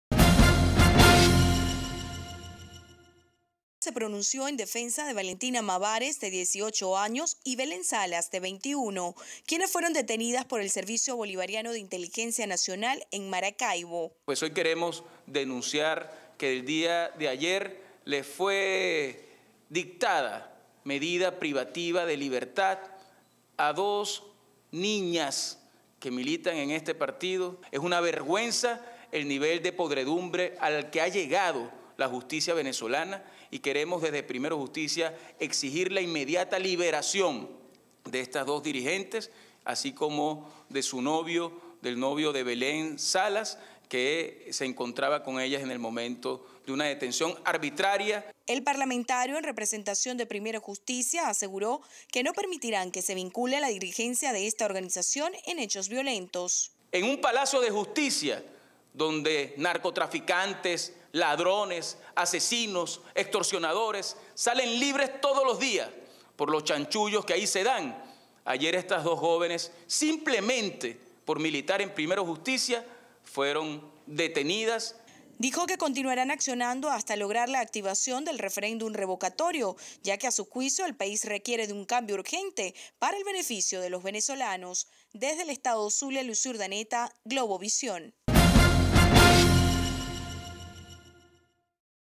Guanipa exige liberación de 2 adolescentes detenidas por el Sebin
El diputado a la Asamblea Nacional (AN) Tomás Guanipa rechazó la privativa de libertad de dos jóvenes que el pasado jueves fueron “secuestradas" por el Servicio Bolivariano de Inteligencia (Sebin), al tiempo que exigió su pronta liberación